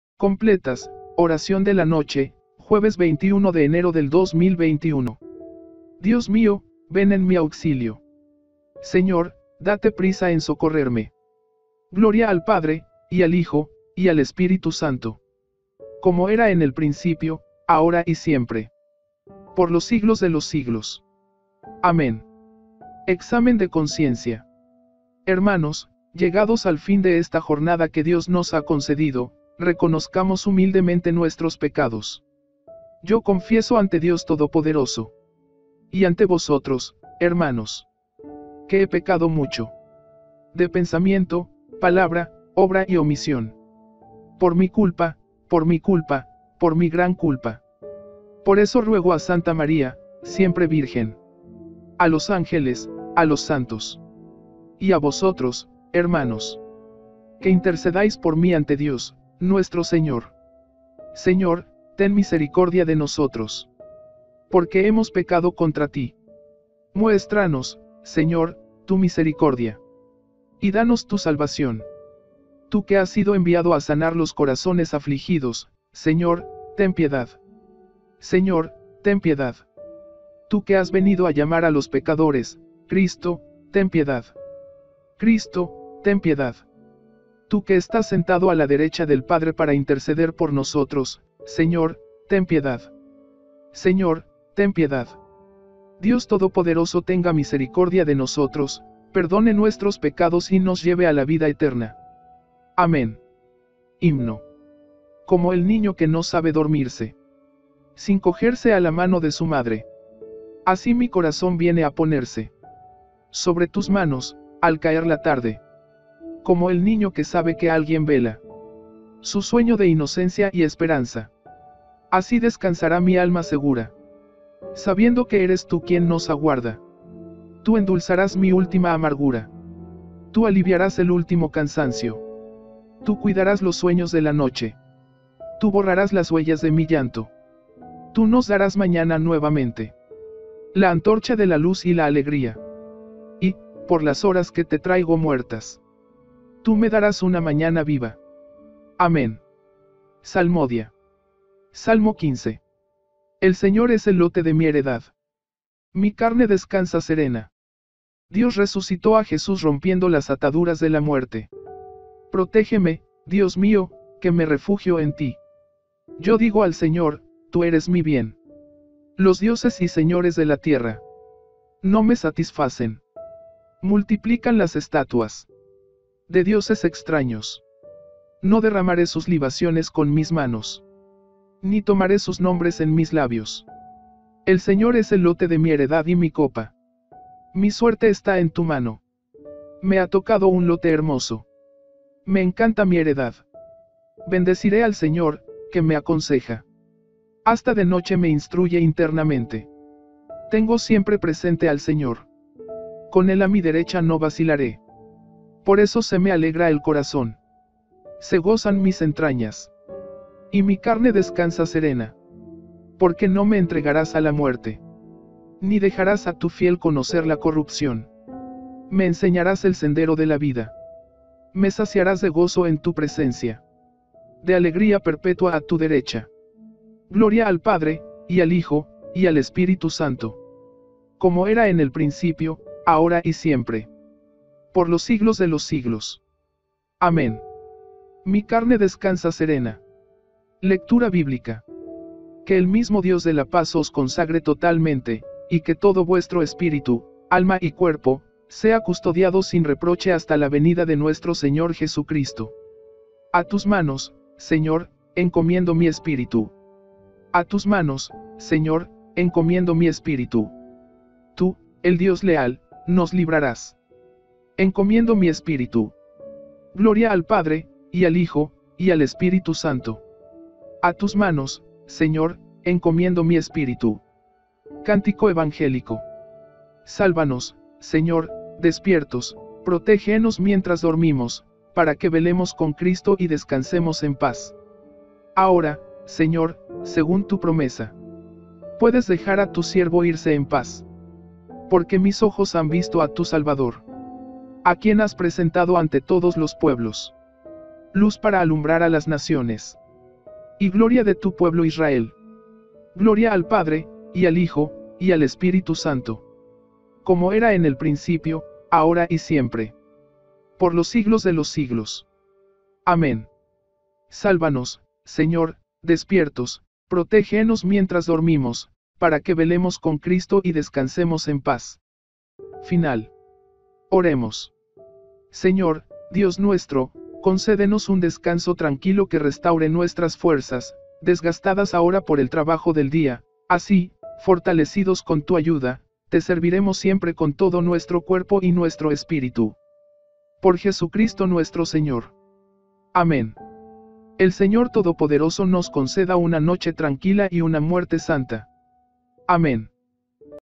COMPLETAS, ORACION DE LA NOCHE, JUEVES 21 DE ENERO DEL 2021
Completas de hoy jueves 21 de enero del 2021.
Una oración de la noche hecha con verdadera fe y con amor profundo a Nuestro Señor, es una hermosa manera de terminar el día. Una forma de comunicarnos con Dios para alcanzar su Gloria y renovar nuestros corazones.
Te animamos a empezar desde hoy a apartar unos minutos de tu día, y establecer una comunicación espiritual con el Creador, quien está presto a escucharnos y a ayudarnos. Solo es necesario dar ese primer paso, donde con una sencilla y bonita oración lográs romper las barreras y decir presente, a un Dios que te ama y te protege.